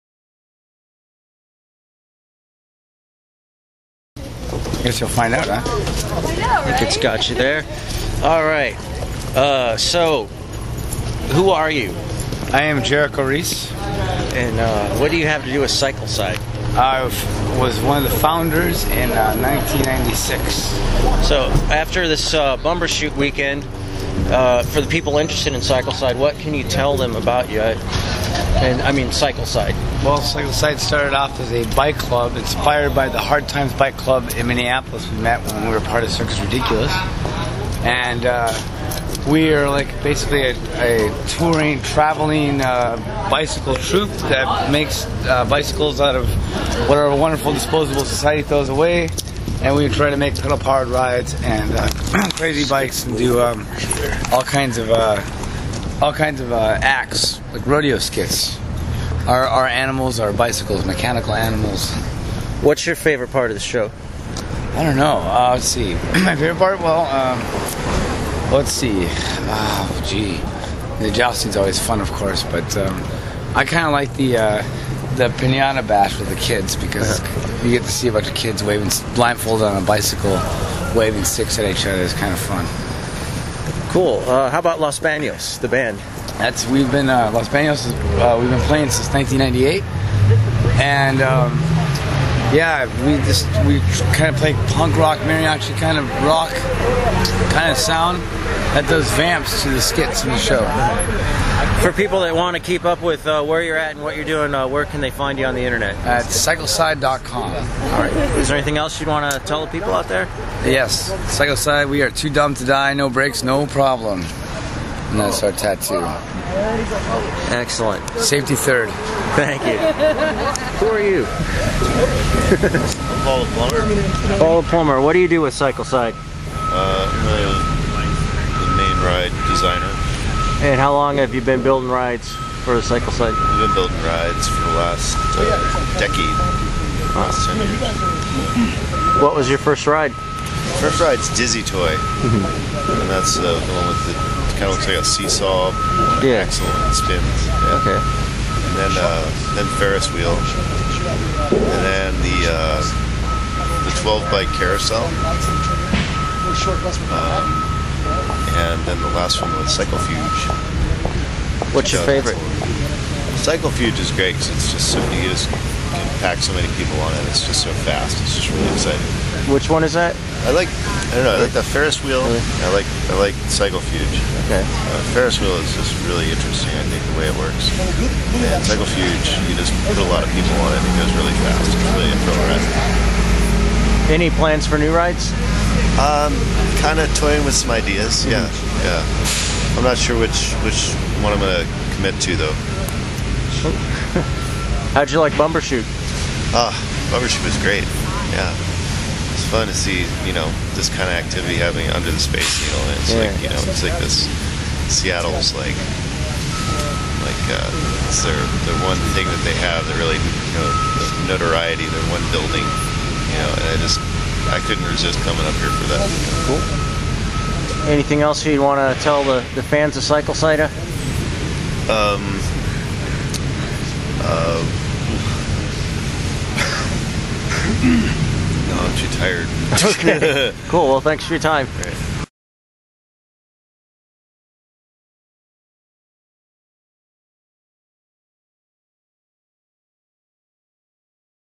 Cyclecide Interview
After Bumbershoot in Seattle 2007, this is an interview with Jarico and Paul the Plumber.